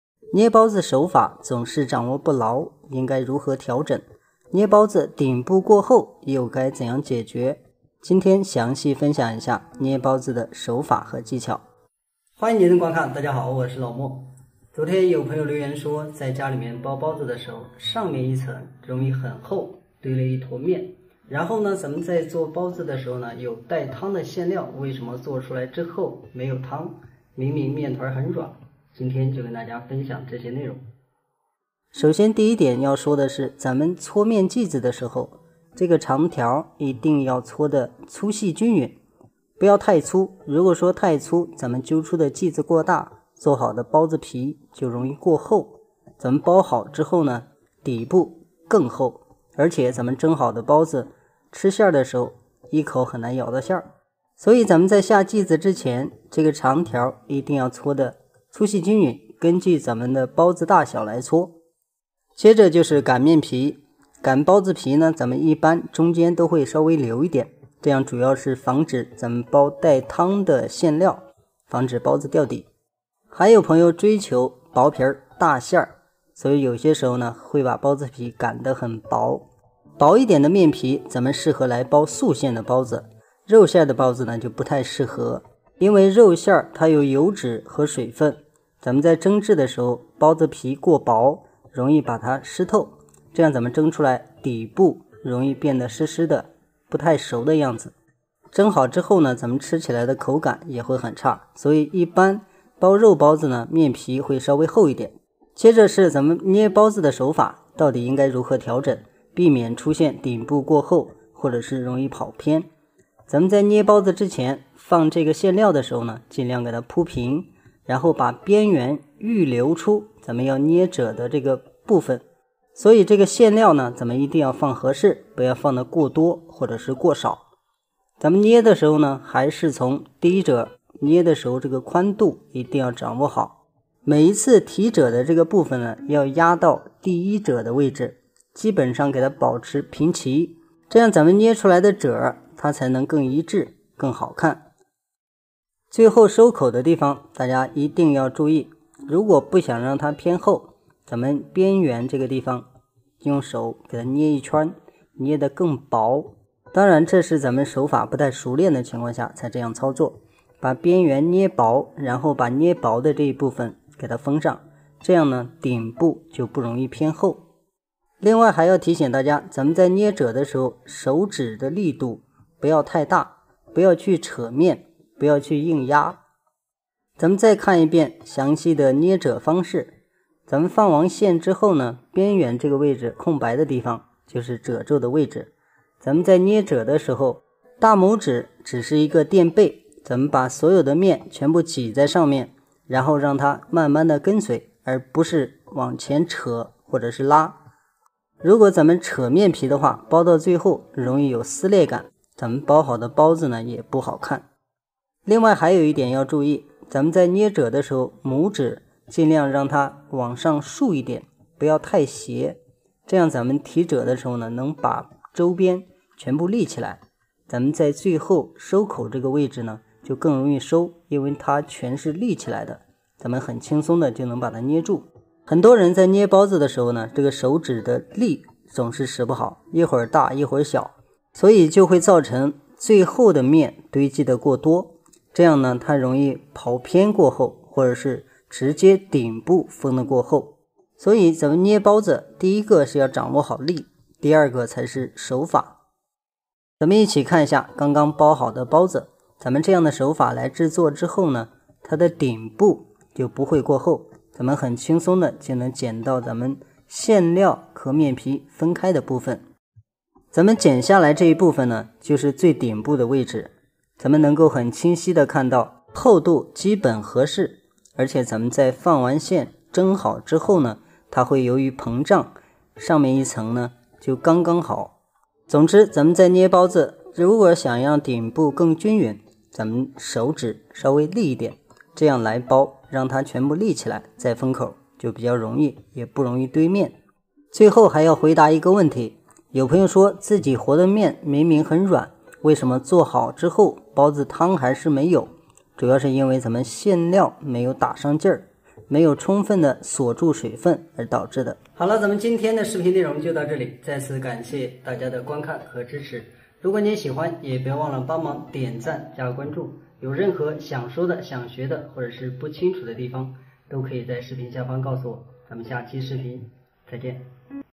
包子收口厚容易斜，肉馅没汤怎么办？面点师教你技巧，轻松改善
专职面点师 面食百晓生 食物探鲜者！！分享真技术，记录真食界
大家好 我是老莫